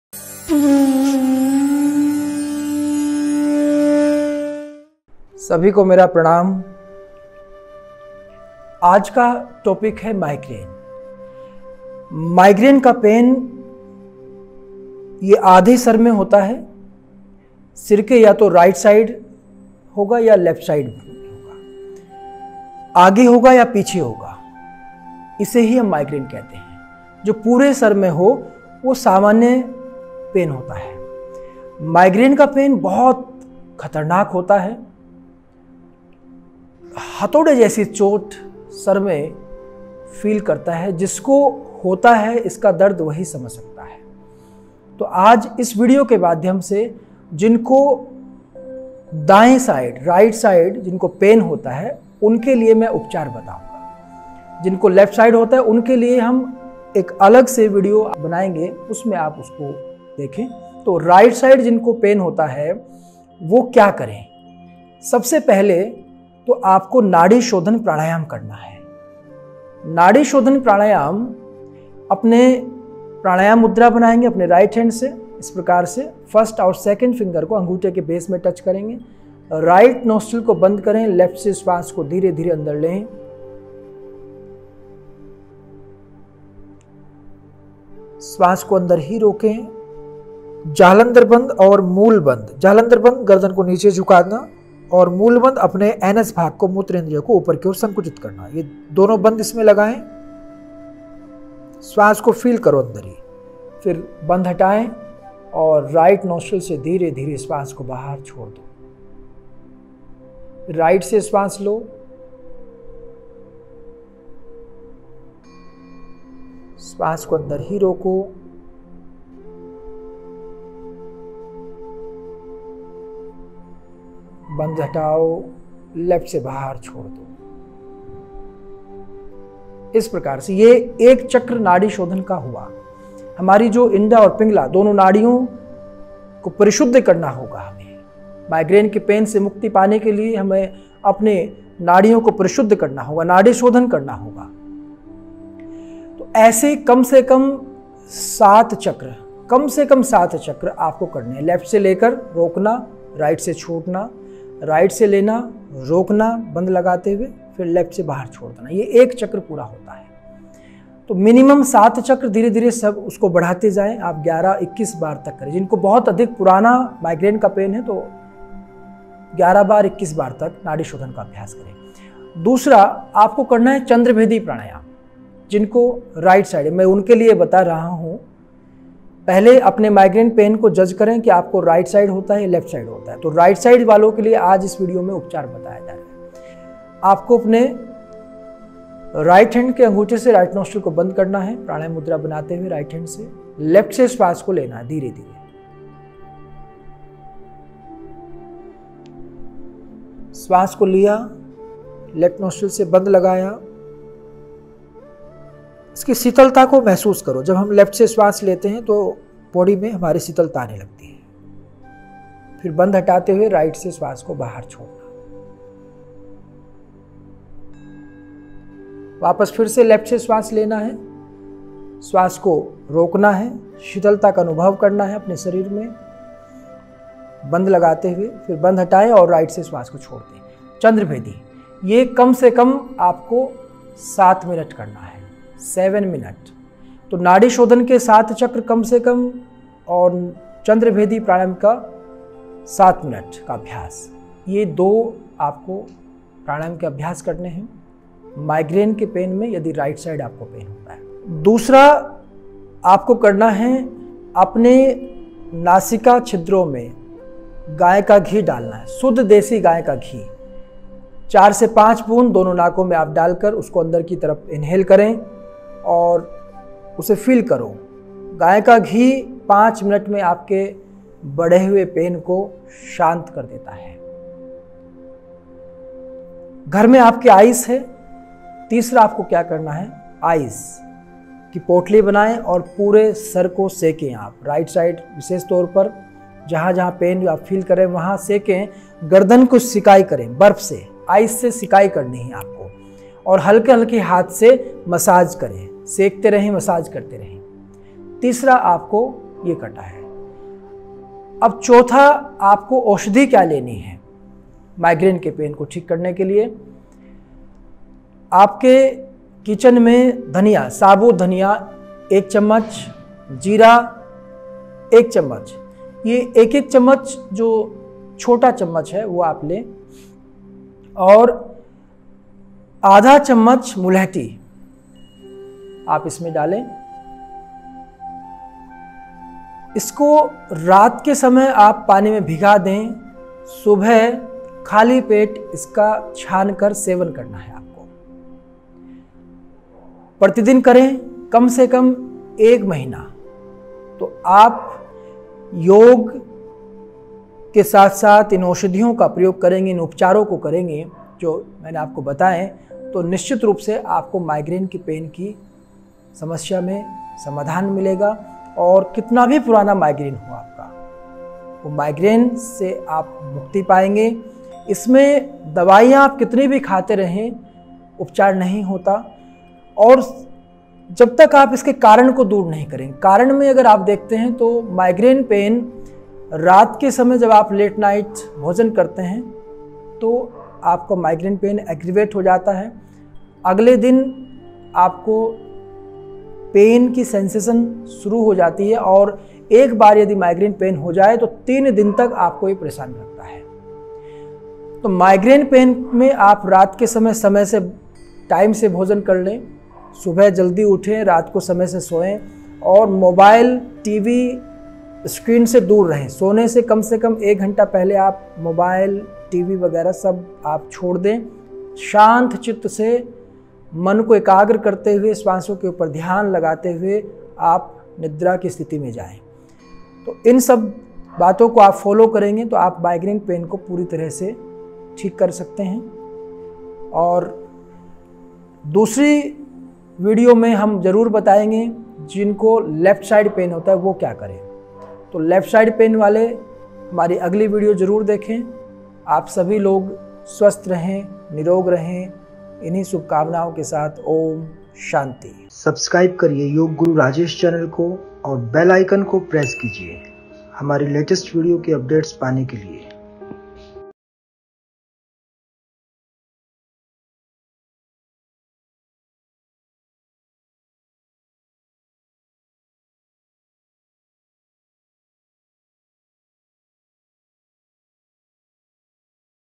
माइग्रेन सिर में दाँयी तरफ़ दर्द रहता है तो यह विडियो आपके लिए हैं ।। Yog Guru Rajesh ।। Aarogyam।।
माइग्रेन पेन यदि दाईं तरफ़ सिर के भाग में दर्द होता है और वह दर्द बहुत तेज़ हो तो समझ लेना चाहिए कि माइग्रेन का दर्द इसके स्थाई समाधान के लिए योग  कारगर है अपने खानपान में कुछ परिवर्तन करके आप इस रोग से मुक्ति पा सकते हैं वीडियो में बताये कुछ तरीक़ों को आप अपनाएंगे तो मैं आसरा करूँगा आप जल्द ही माइग्रेन के दर्द से मुक्ति पायेंगे ।